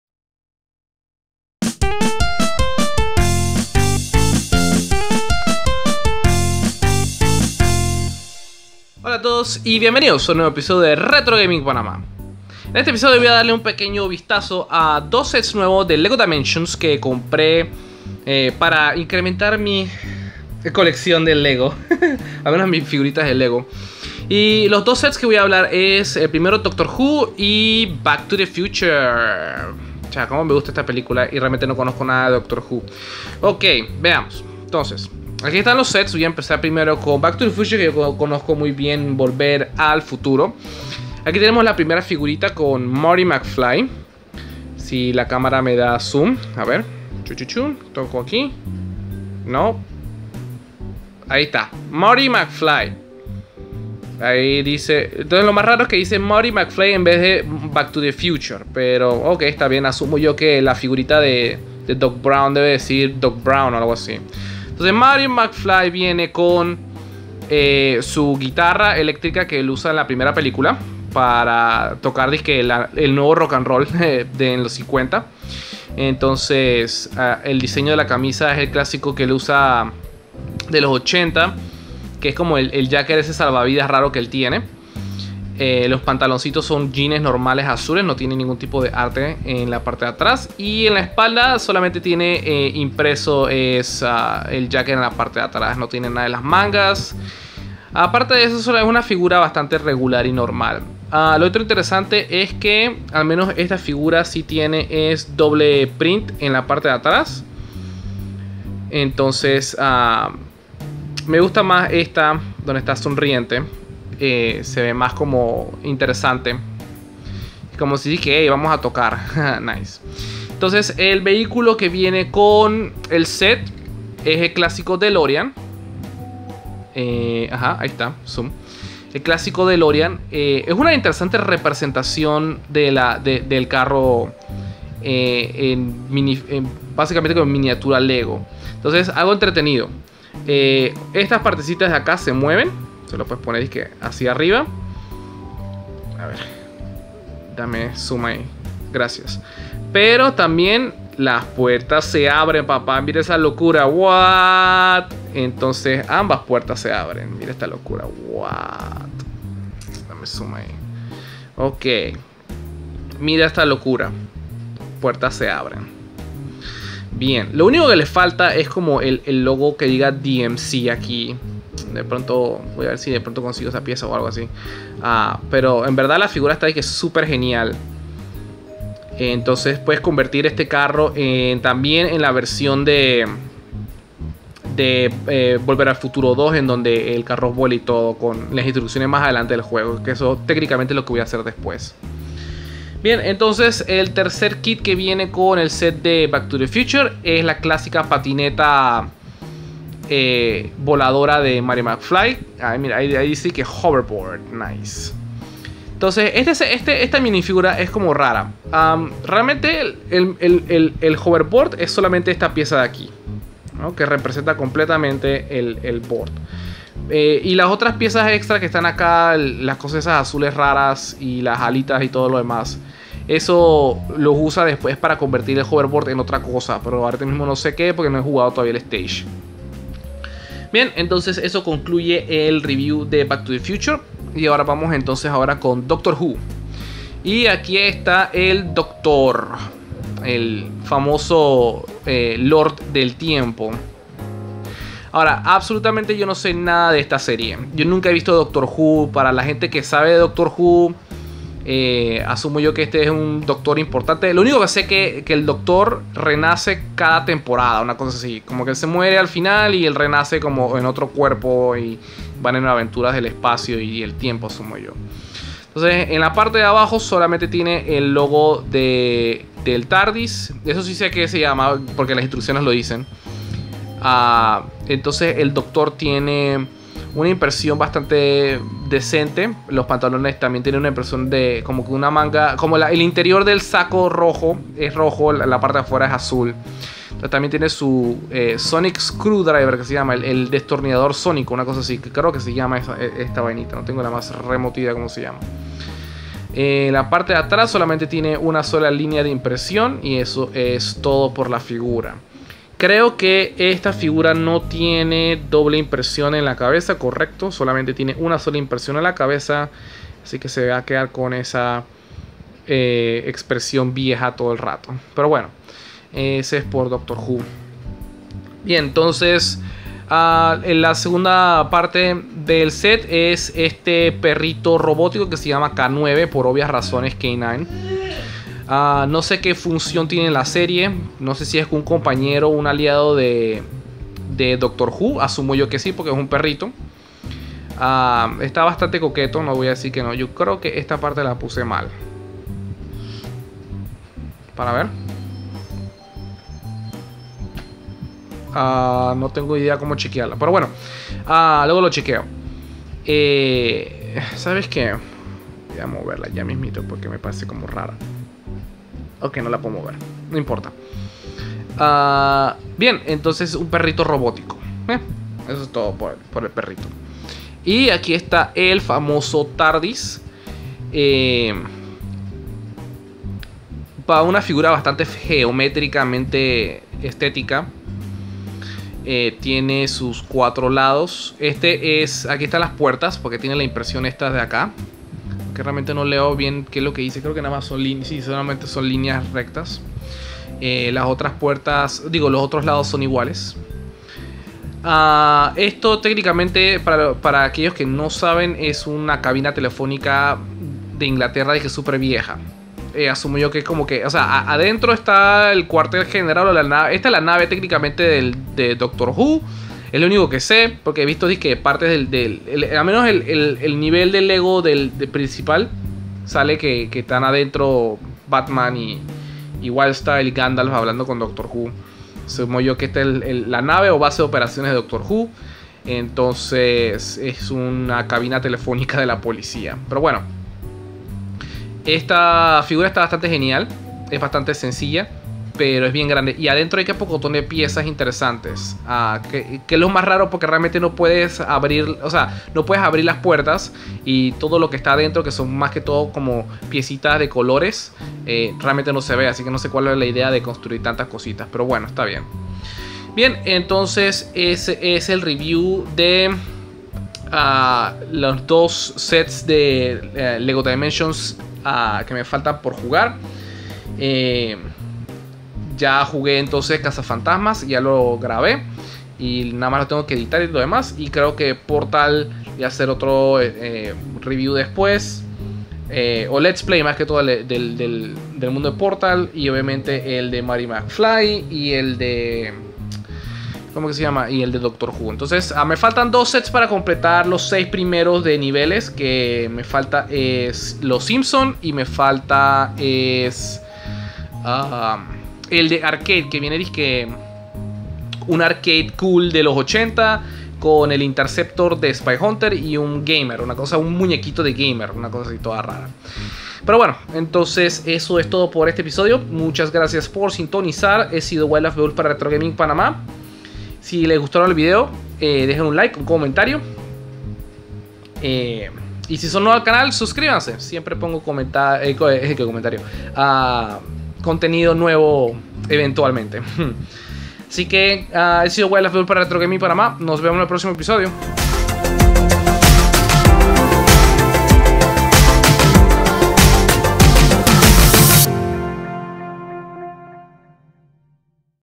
2 nuevos sets de Lego Dimensions - Back to to the Future y Doctor Who
2 nuevos sets para el juego de Lego Dimensions. Back to the Future y Doctor Who.